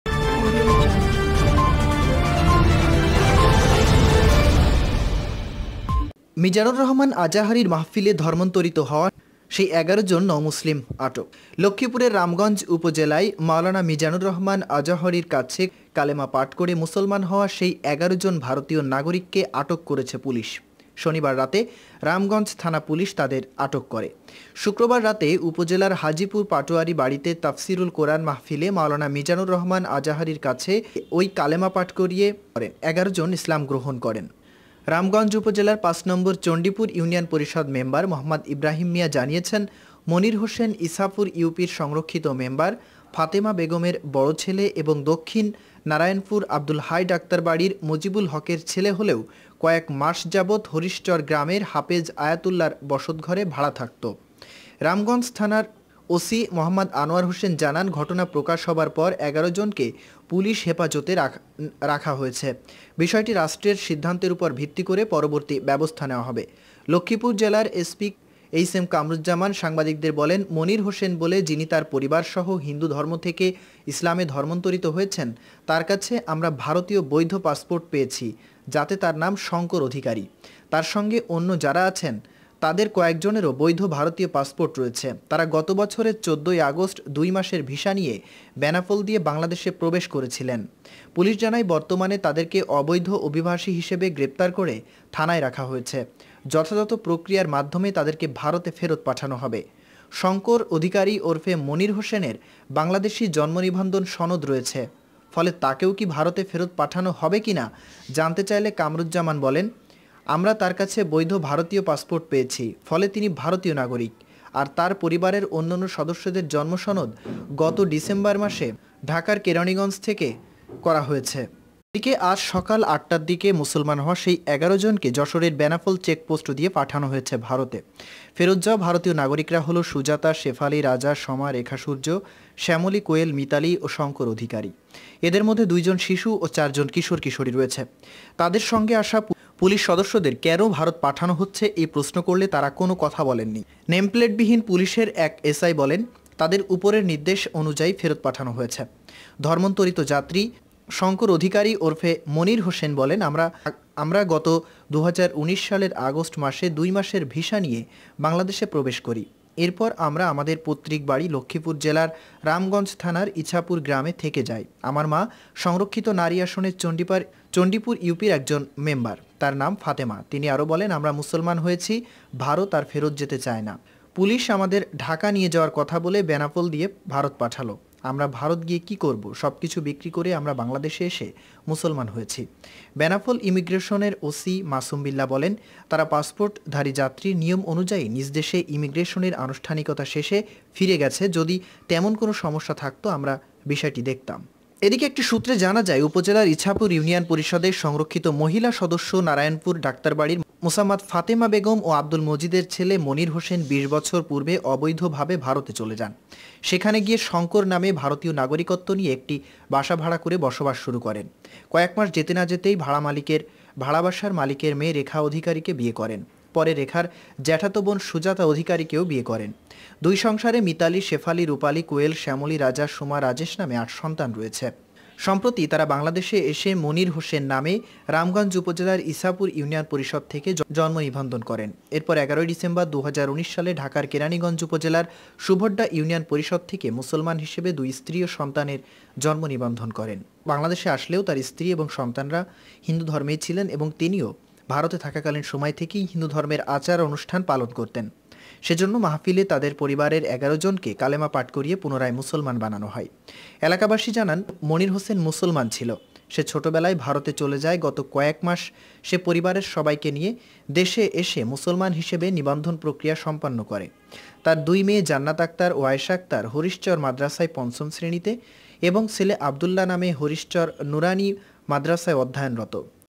যেকারণে আজহারীর মাহফিলে মুসলিম হওয়া সেই ১১ জনকে আটক করে ভারত পাঠাল
লক্ষ্মীপুরের রামগঞ্জ উপজেলায় মাওলানা মিজানুর রহমান আজহারীর কাছে কালেমা পাঠ করে মুসলমান হওয়া সেই ১১ জন ভারতীয় নাগরিককে আটক করেছে পুলিশ।

শনিবার রাতে রামগঞ্জ থানা পুলিশ তাদের আটক করে।

শুক্রবার রাতে উপজেলার হাজীপুর পাটোয়ারীর বাড়িতে তাফসিরুল কোরআন মাহফিলে মাওলানা মিজানুর রহমান আজহারীর কাছে ওই কালেমা পাঠ করিয়ে ওই ১১ জন ইসলাম গ্রহণ করেন।